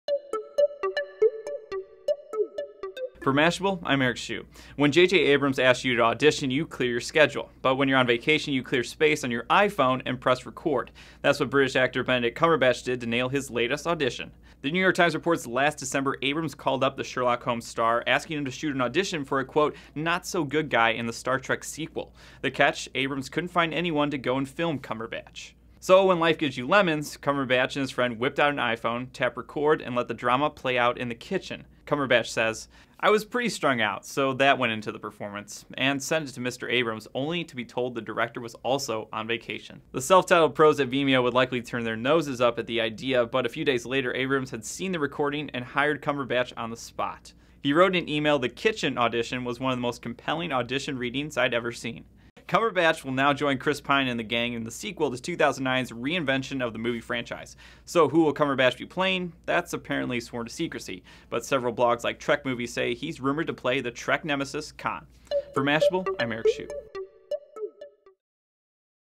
Actor Uses iPhone to Nail Audition for JJ Abrams' Star Trek Sequel
When JJ Abrams asks you to audition -- you clear your schedule. But when you're on vacation, you clear space on your iPhone and press record. That's what British actor Benedict Cumberbatch did to nail his last audition.